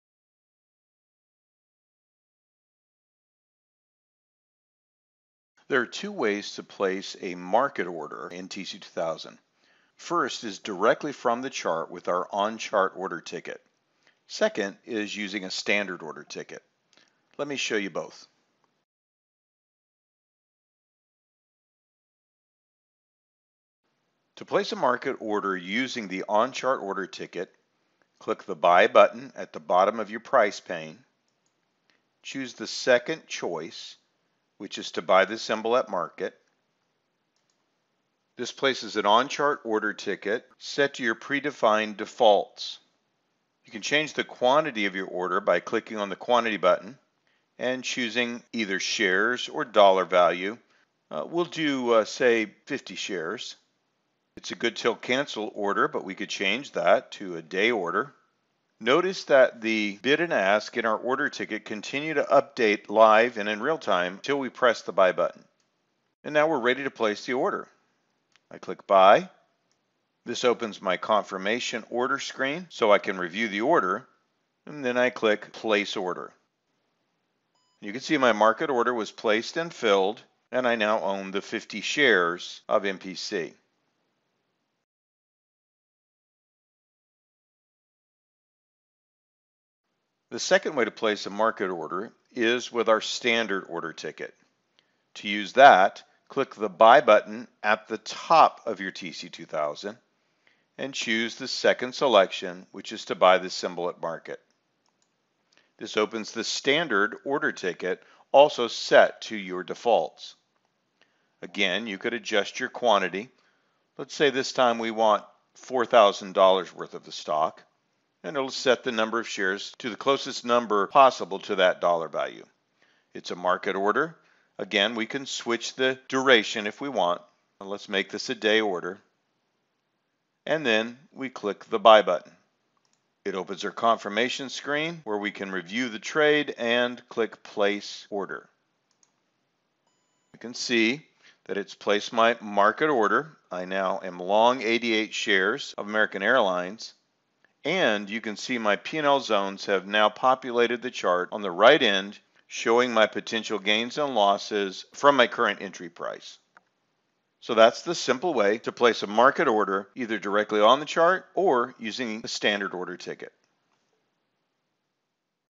TC2000: How to Buy a Stock Using a Market Order
There are several ways to use the TC2000 trading software to place a market order. One of the two methods is by placing an on-chart order ticket. The other option is to create a standard order ticket.